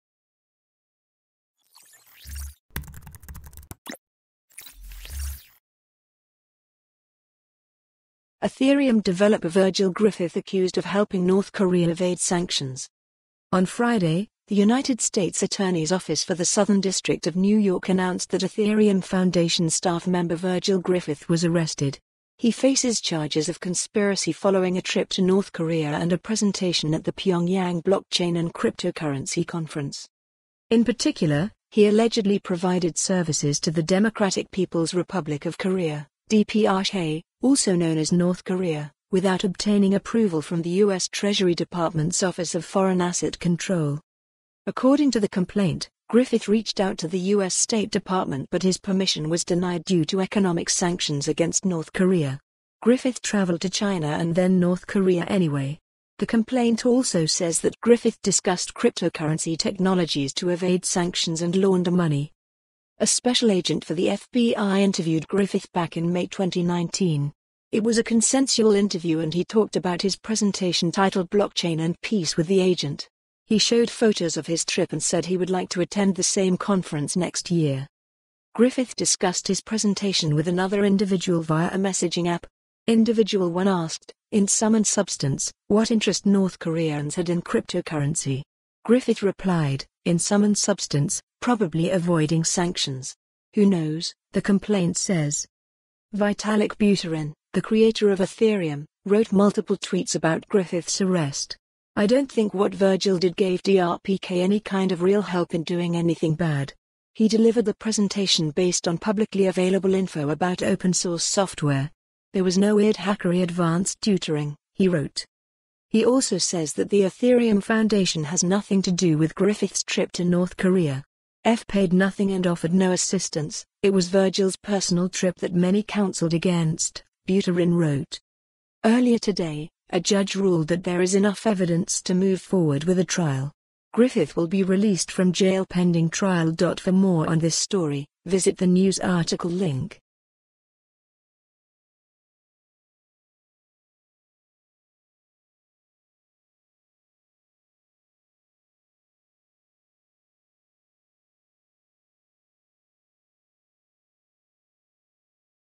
Ethereum developer Virgil Griffith accused of helping North Korea evade sanctions
On Friday, the United States Attorney's Office for the Southern District of New York announced that Ethereum Foundation staff member Virgil Griffith was arrested. He faces charges of conspiracy following a trip to North Korea and a presentation at the Pyongyang Blockchain and Cryptocurrency Conference.

In particular, he allegedly provided services to the Democratic People's Republic of Korea (DPRK, also known as North Korea) without obtaining approval from the U.S. Treasury Department's Office of Foreign Asset Control.

According to the complaint, Griffith reached out to the U.S. State Department but his permission was denied due to economic sanctions against North Korea. Griffith traveled to China and then North Korea anyway. The complaint also says that Griffith discussed "cryptocurrency technologies to evade sanctions and launder money."

A special agent for the FBI interviewed Griffith back in May 2019. It was a consensual interview and he talked about his presentation titled "Blockchain and Peace" with the agent. He showed photos of his trip and said he would like to attend the same conference next year.

Griffith discussed his presentation with another individual via a messaging app. "Individual-1 asked, in sum and substance, what interest North Koreans had in cryptocurrency. Griffith replied, in sum and substance, 'probably avoiding sanctions… who knows,'" the complaint says.

Vitalik Buterin, the creator of Ethereum, wrote multiple tweets about Griffith's arrest. "I don't think what Virgil did gave DRPK any kind of real help in doing anything bad. He *delivered a presentation based on publicly available info about open-source software*. There was no weir